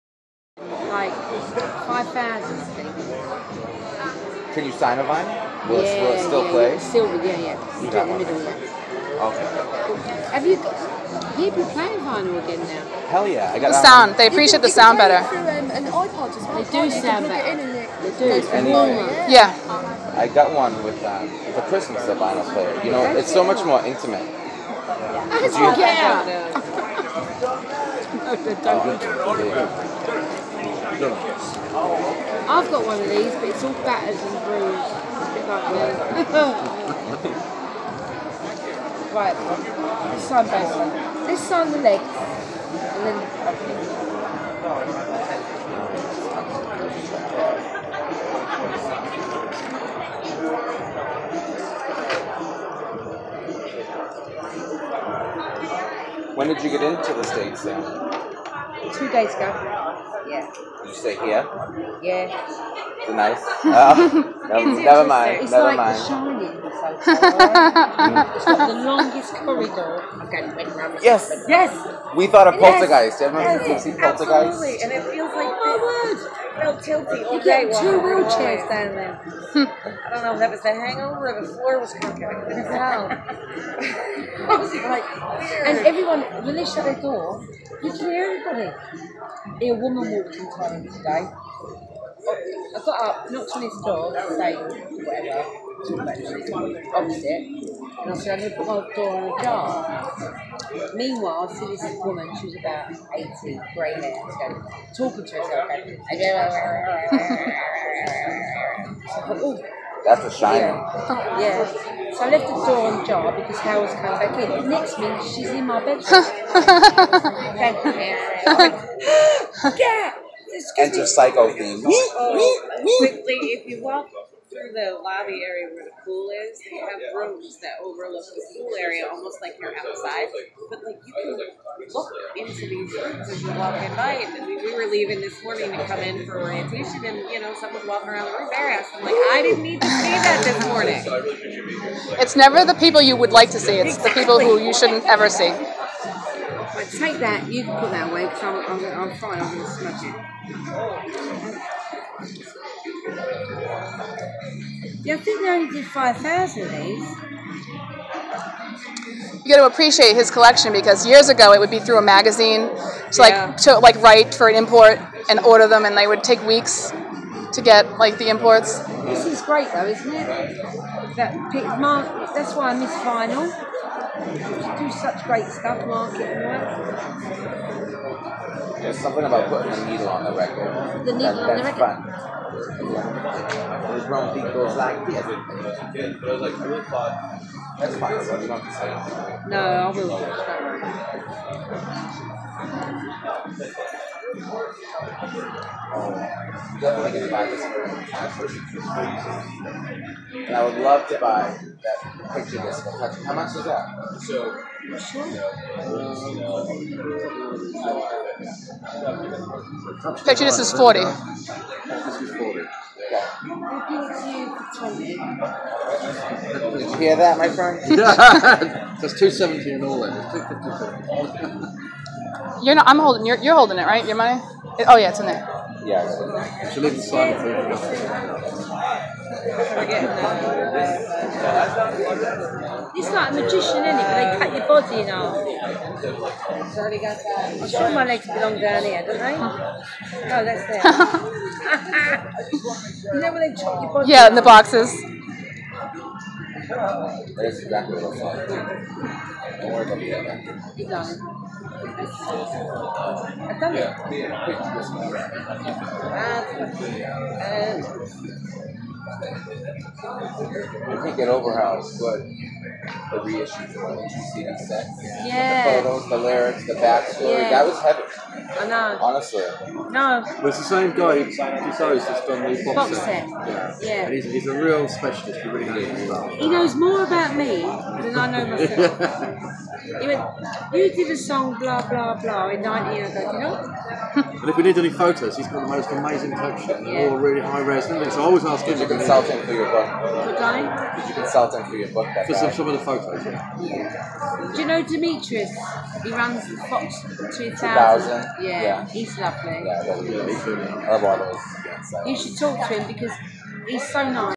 SAMANTHA FOX meet and greet and autographs!❤
Meeting my #1 lady and icon Samantha Fox at Music-Con May 2017 U.S.A video #4